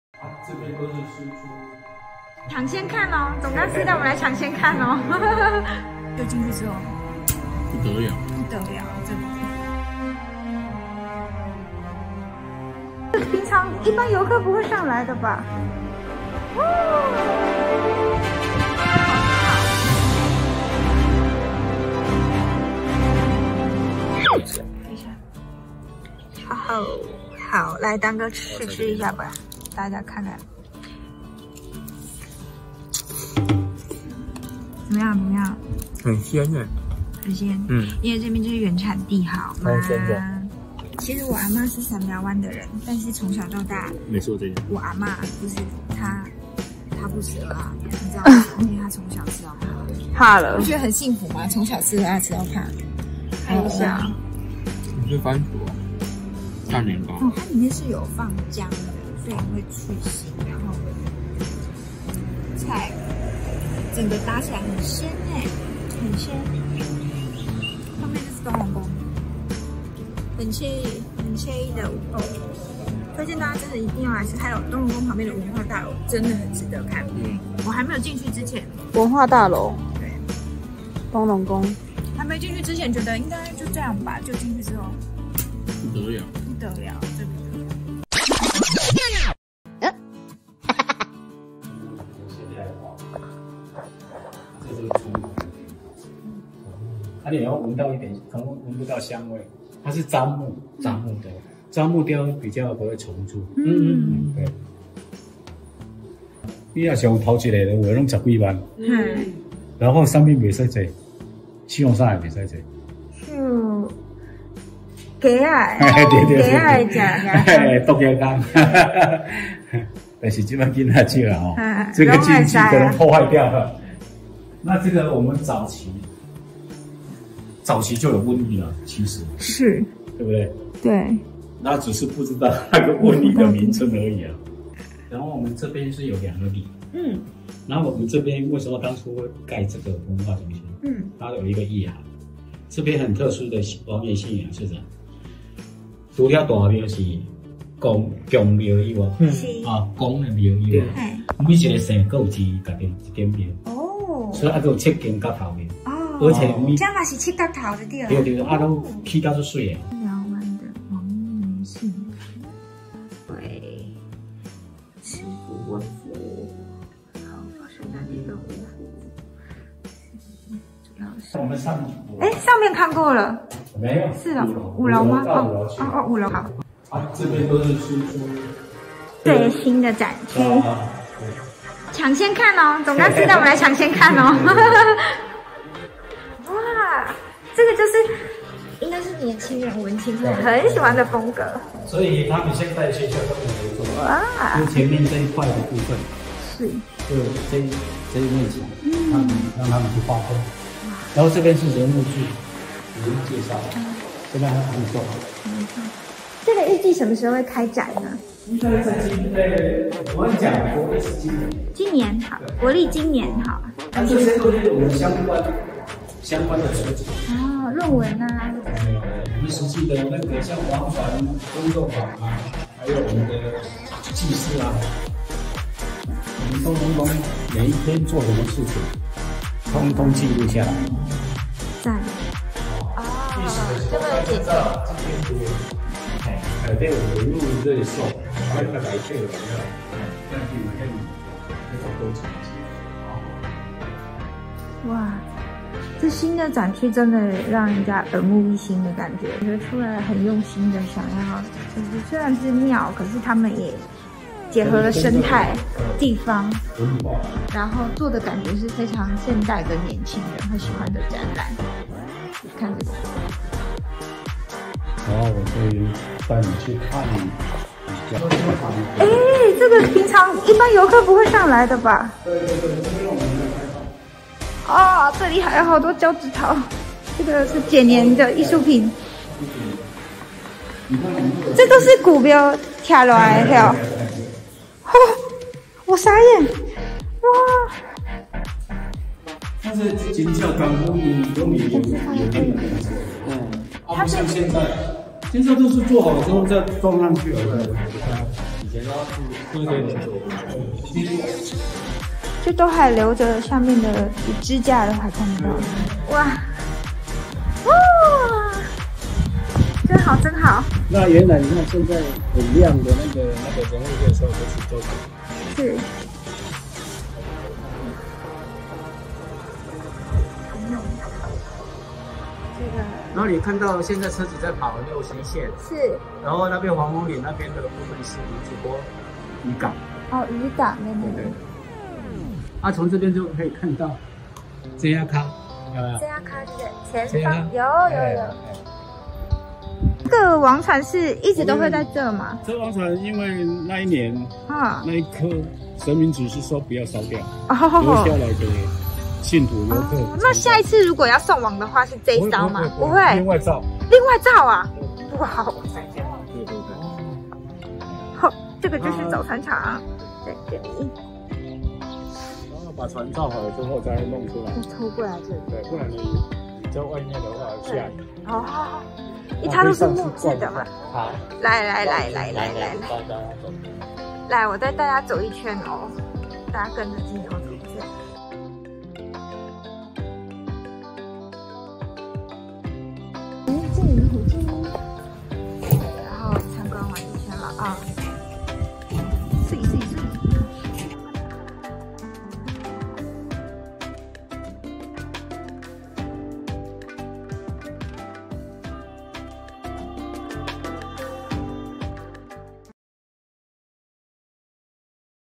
【下港人誒vlog】 開箱三寮灣東隆宮最新展區
原本我們只是想去拜拜而已啦
沒想到爺爺們竟然安排總幹事來為我們導覽
真是受寵若驚呀~

這次總幹事還帶我們參觀尚未開放的最新展區
話不多說，快來跟我們一起看看吧！

● 下港人文化工作室（一步就出走、廟會小學堂）出品 ●